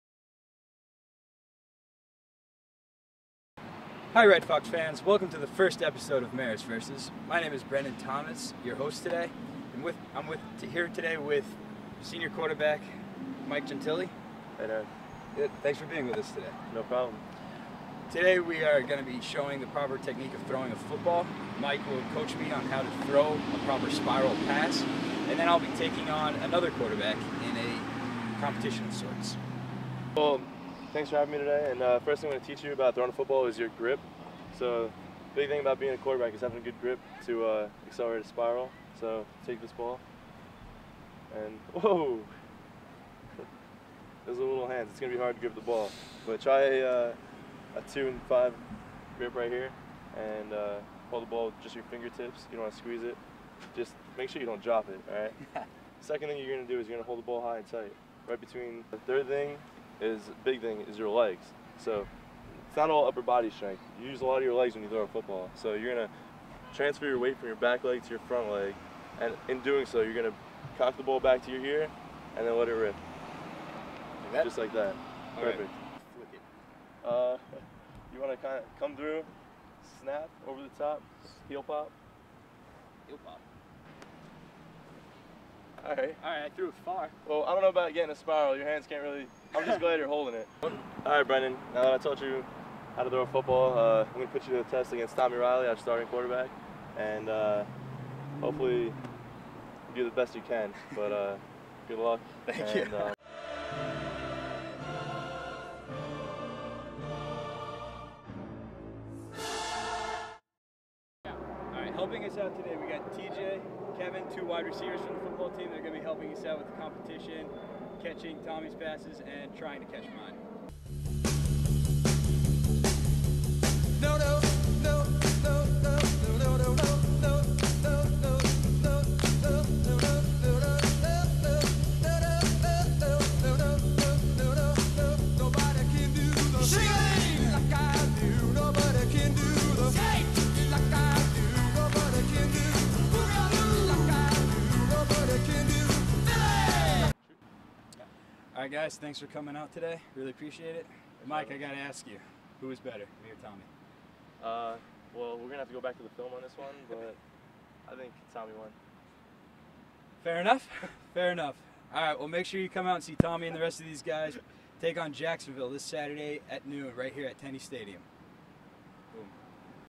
Marist Vs. Episode 1 (Quarterbacks)
First episode of video series meant to promote Marist College athletics and student-athletes